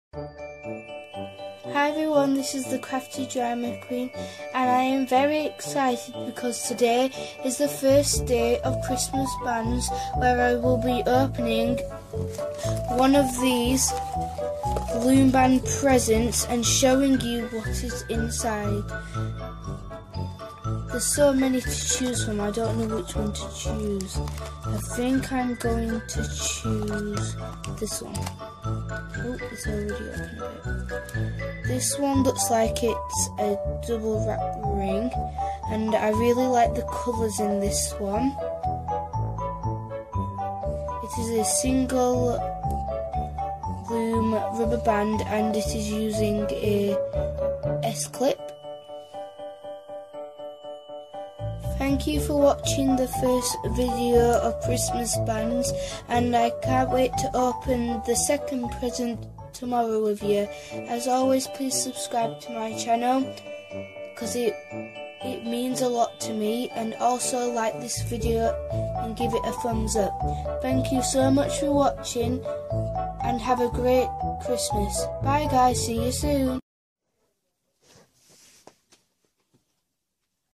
Christmas Bands~Count Down To Christmas~Crafty DramaQueen
Hey guys i hope you enjoy the very 1st day of Christmas Bands where I post a video every day and show you what sort of loom band creation is in the present please understand if I don't post that day I PROMISE you I will post it the next.